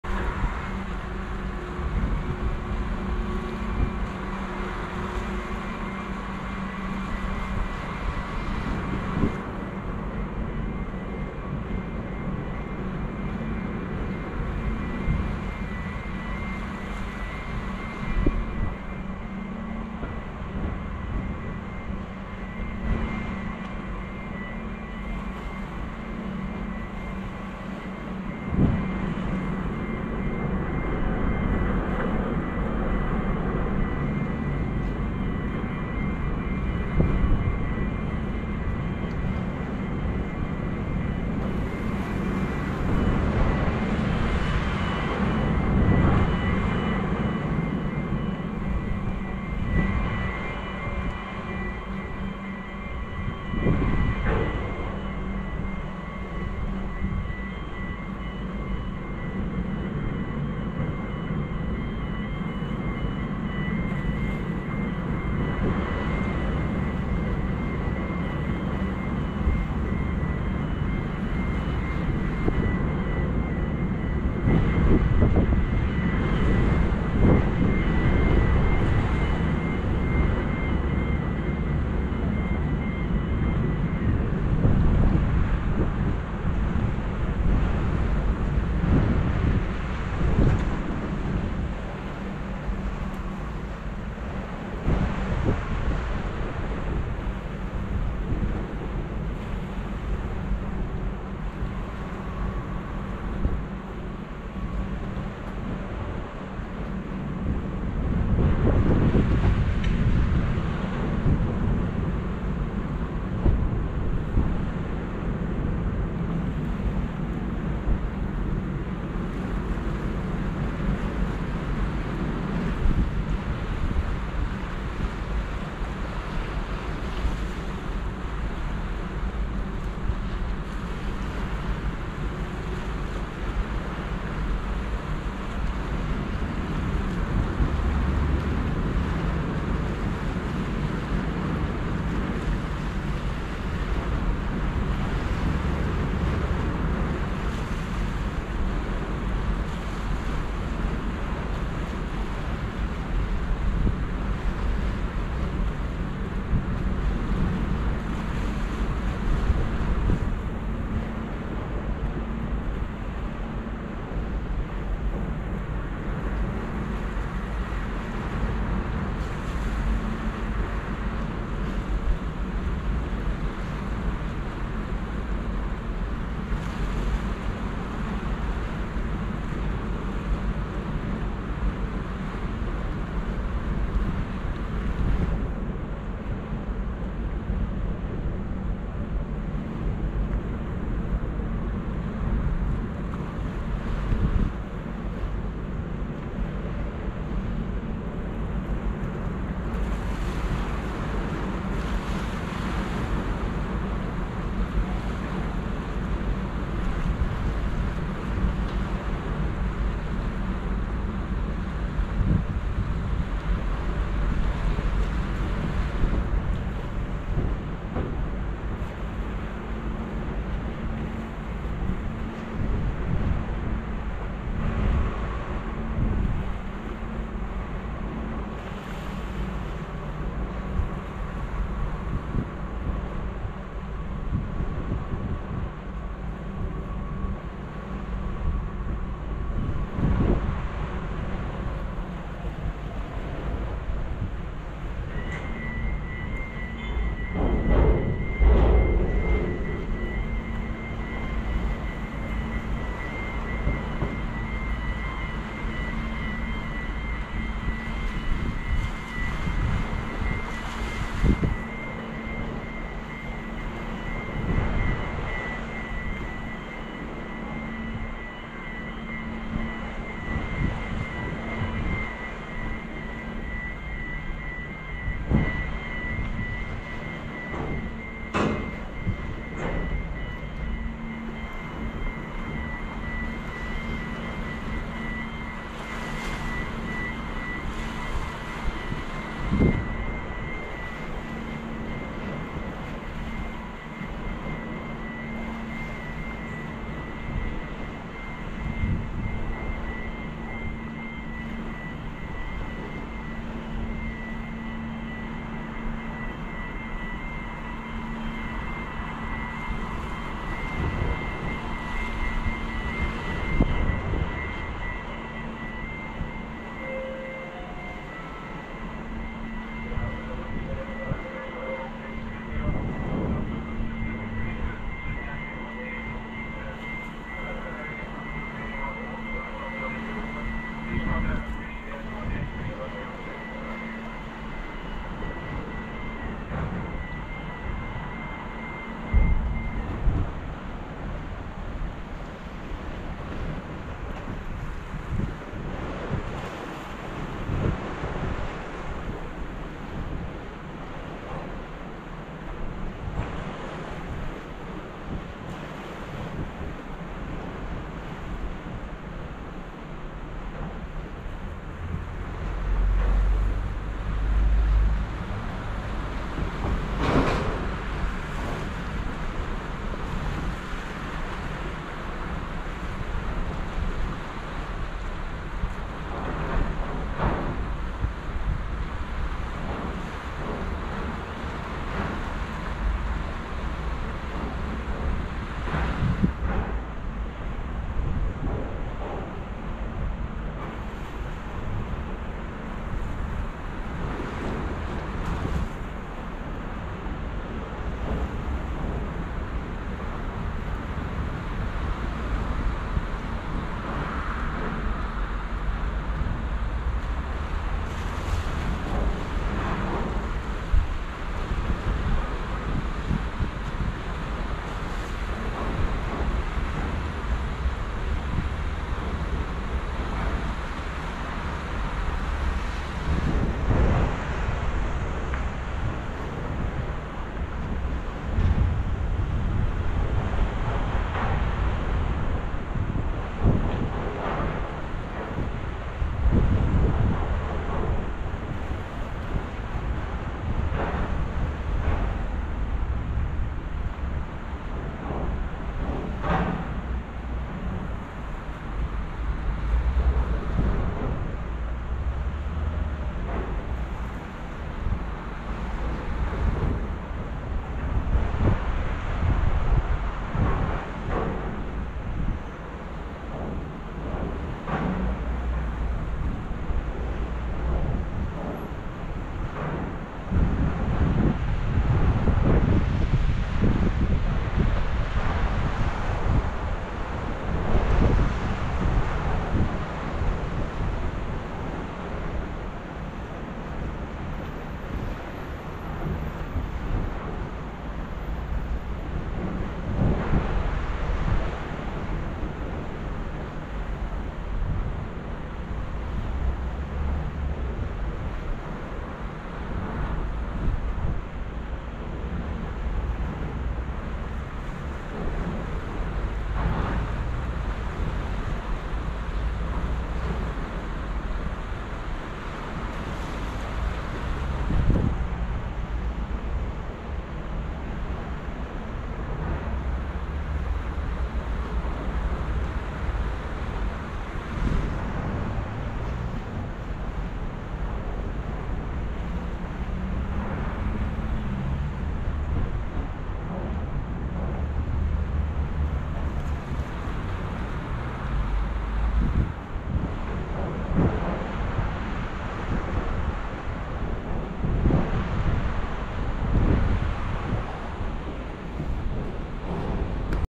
Isle of Mull Ferry docking .
Thought I would just upload the ferry coming in to Mull without editing.